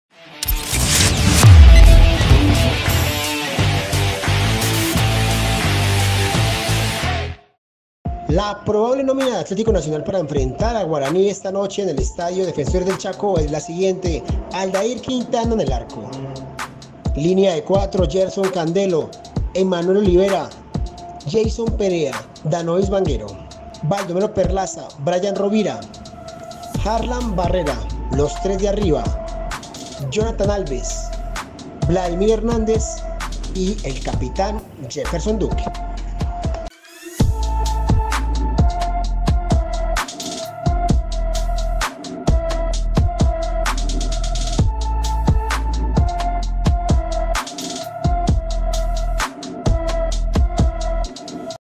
🔥🚨POSIBLE NÓMINA TITULAR DE ATLÉTICONACIONAL PARA EL DEBUT DE LA COPA LIBERTADORES ANTE GUARANÍ🚨🔥
Esta es la posible nómina titular de AtléticoNacional​​​​​​ para enfrentar esta noche a Club Guaraní en el estadio Defensores del Chaco por el partido de ida de la segunda fase de la CopaLibertadores de América 2021.